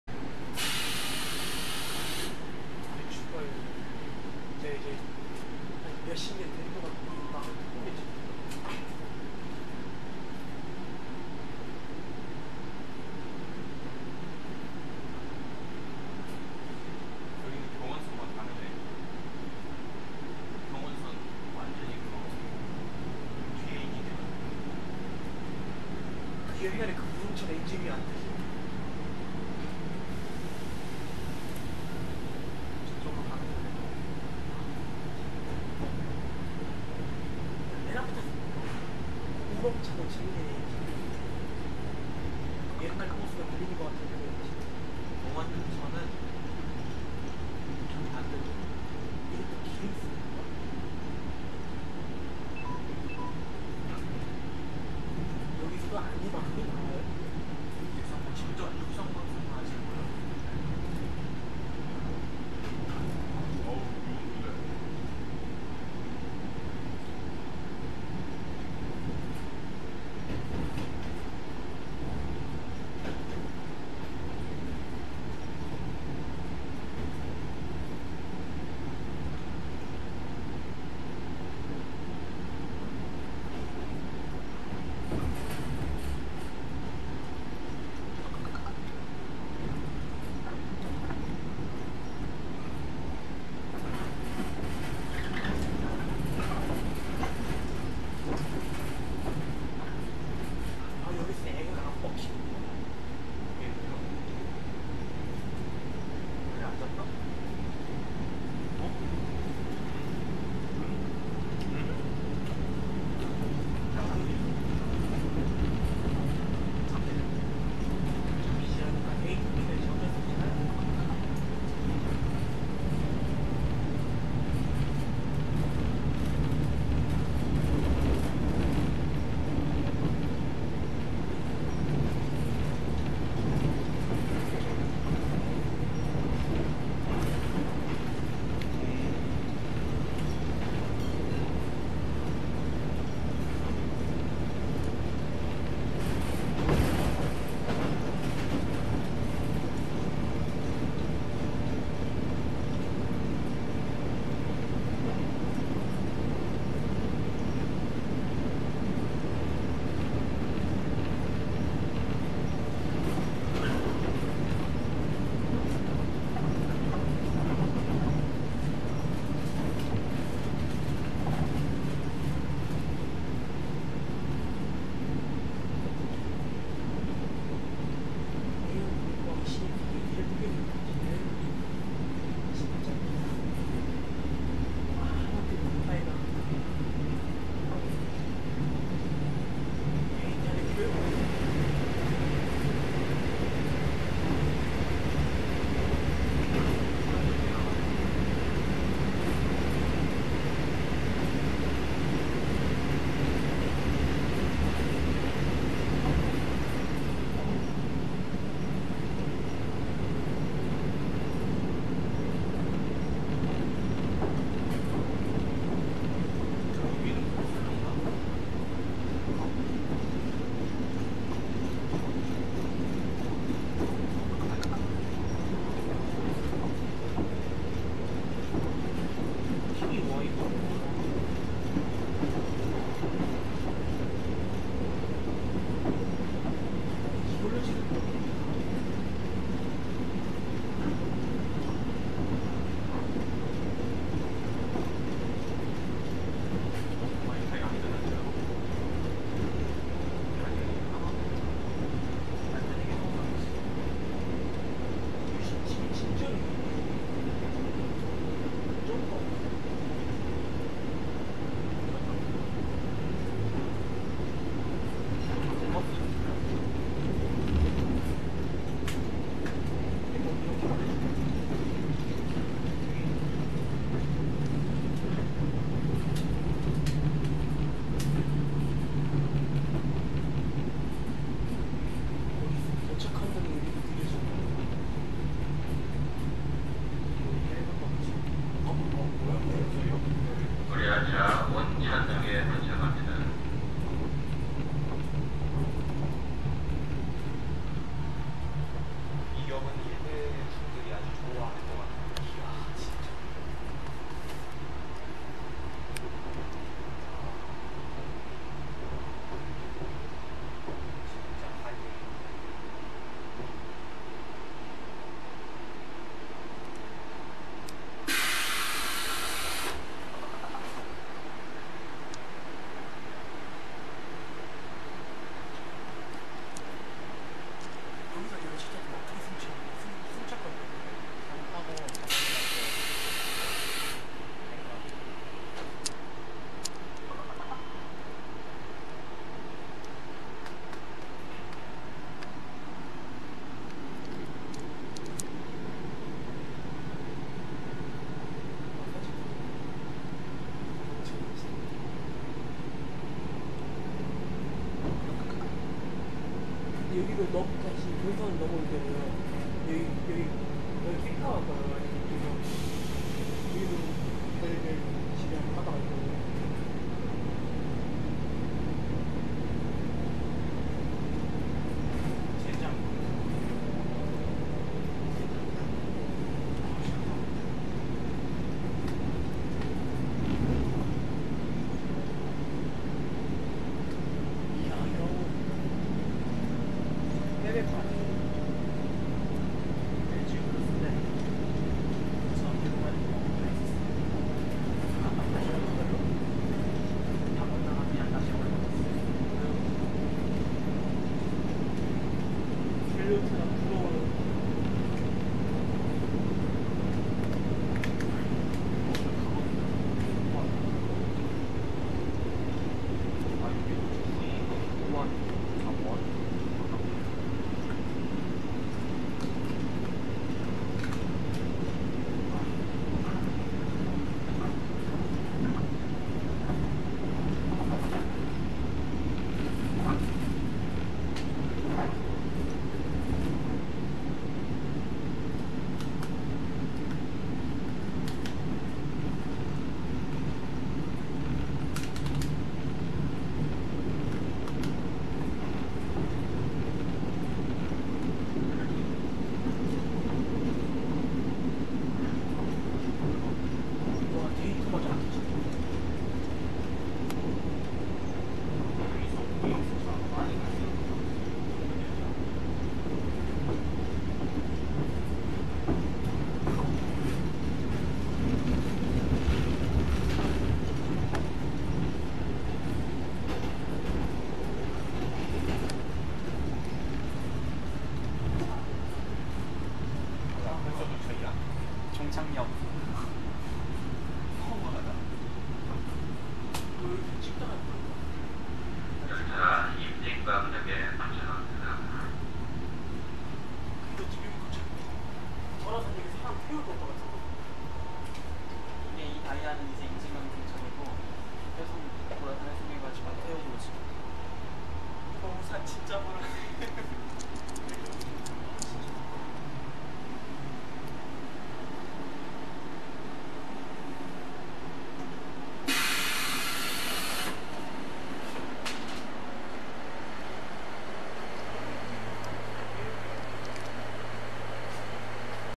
좌측 창문에서 본 경의선 통근열차 운행영상 : 문산역-운천역-임진강역
2013년 7월 10일/제 2709열차